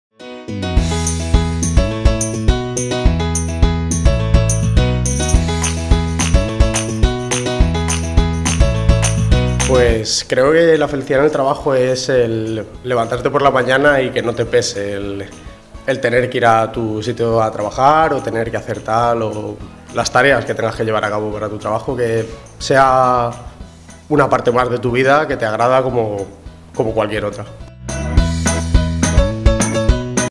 El momento feliz de Miguel Ángel Arquero
Miguel Ángel Arquero, Fundador de la Plataforma Cramway E-learning, nos explica qué es para él la felicidad en el trabajo.